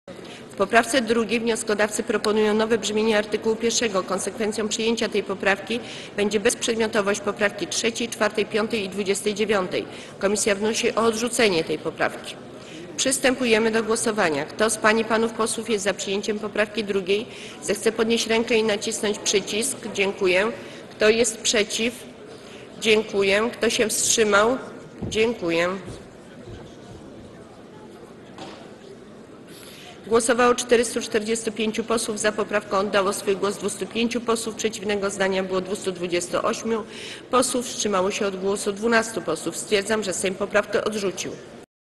[22/301] Ewa Kopacz: W poprawce 2. wnioskodawcy proponują nowe brzmienie art. 1. Konsekwencją pr..
Posiedzenie Sejmu nr 9
2 marca 2012 r. 09:23:01
Marszałek Ewa Kopacz:

W poprawce 2.  wnioskodawcy proponują nowe brzmienie art.  1.  Konsekwencją przyjęcia tej poprawki będzie bezprzedmiotowość poprawek 3., 4., 5.  i 29.

Komisja wnosi o odrzucenie tej poprawki.  Przystępujemy do głosowania.   Kto z pań i panów posłów jest za przyjęciem poprawki 2., zechce podnieść rękę i nacisnąć przycisk.  Kto jest przeciw?  Kto się wstrzymał?

Głosowało 445 posłów.  Za poprawką oddało głos 205 posłów, przeciwnego zdania było 228 posłów, wstrzymało się od głosu 12 posłów.  Stwierdzam, że Sejm poprawkę odrzucił.